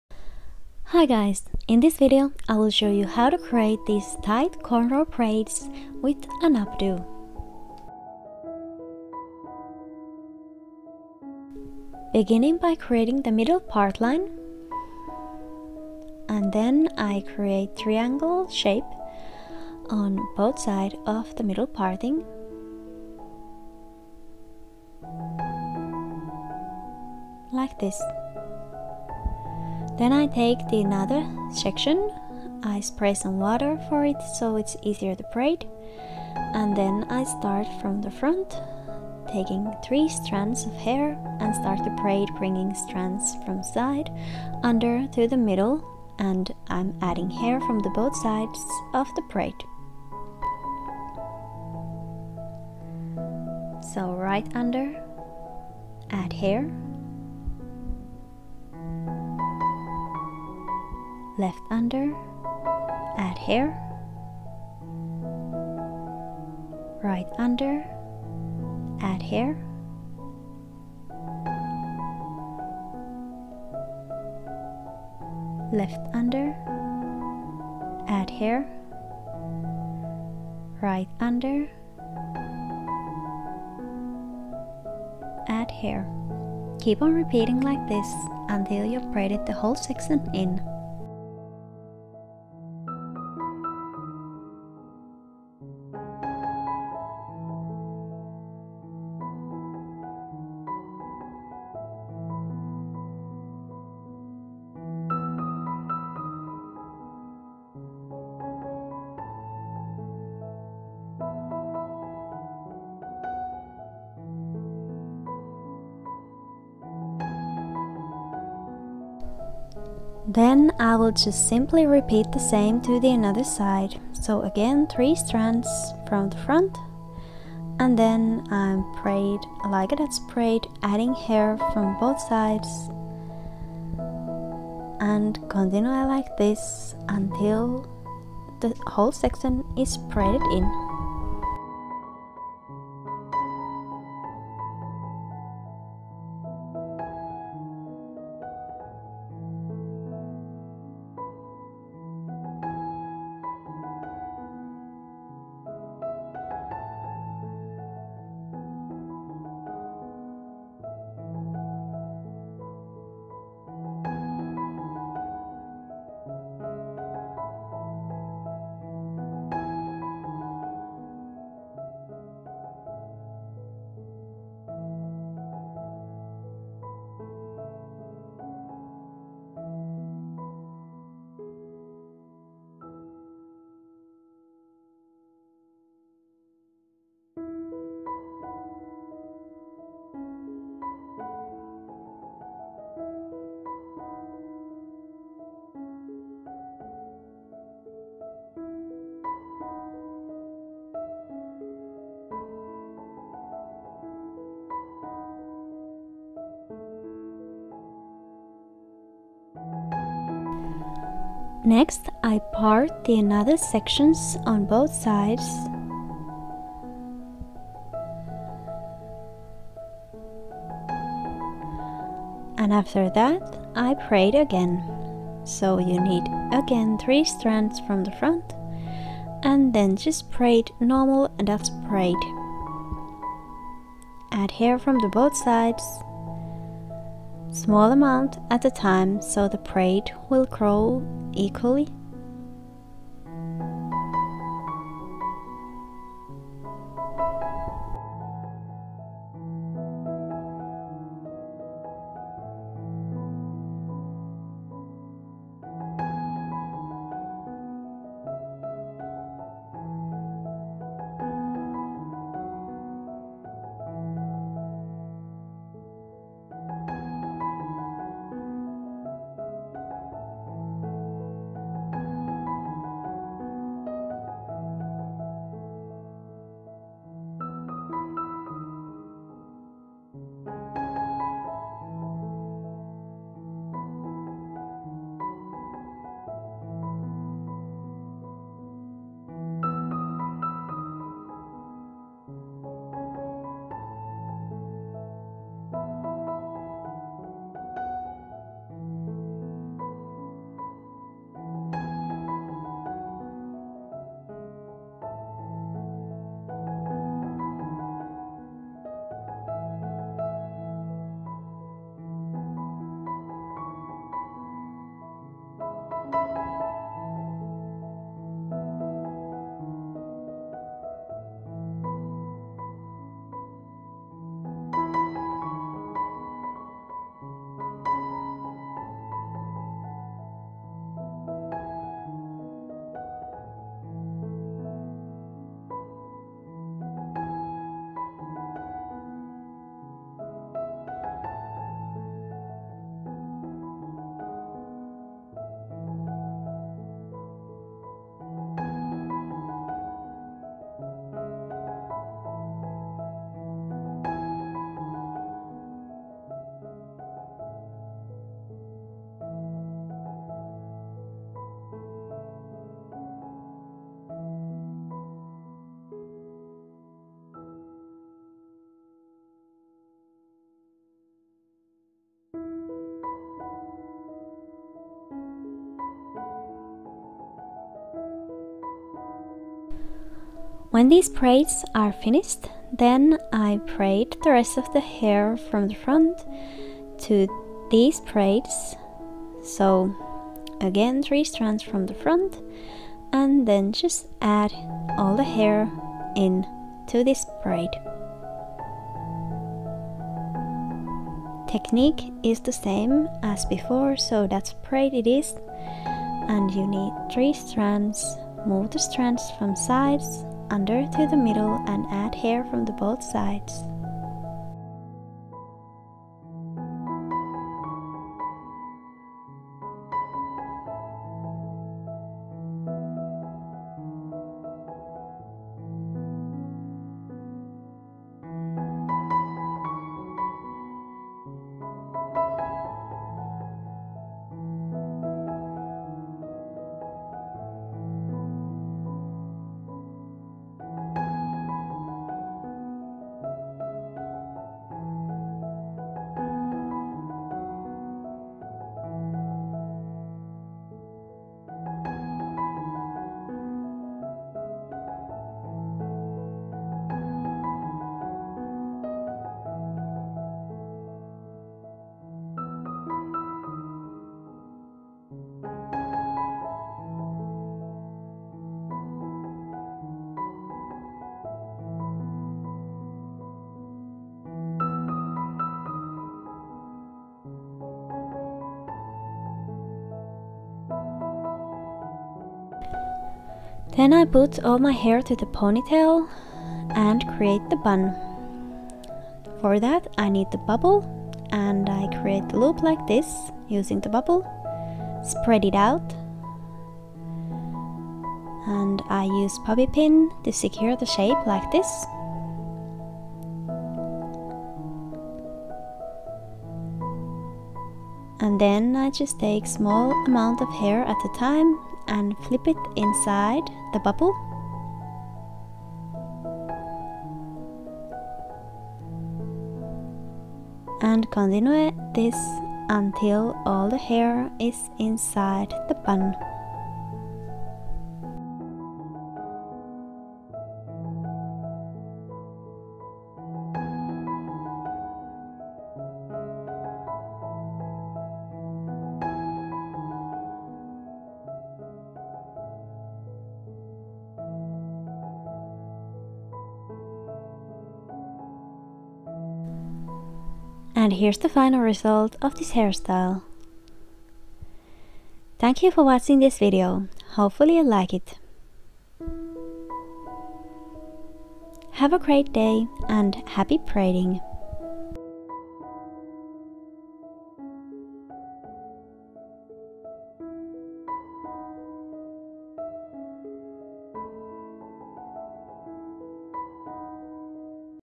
Tight Cornrow Braids for Updo | Easy Hairstyles for Long Hair
Triangle cornrow braids with messy updo! All you need is dutch braids for these cornrows and messy bun style! This is nice updo for daily lifestyle, hobbies, for sport, ballet hair or for gymnastics and it last easily many days! Braided hairstyles look gorgeous and keep the long hair away from the face! This hairstyle is for long hair.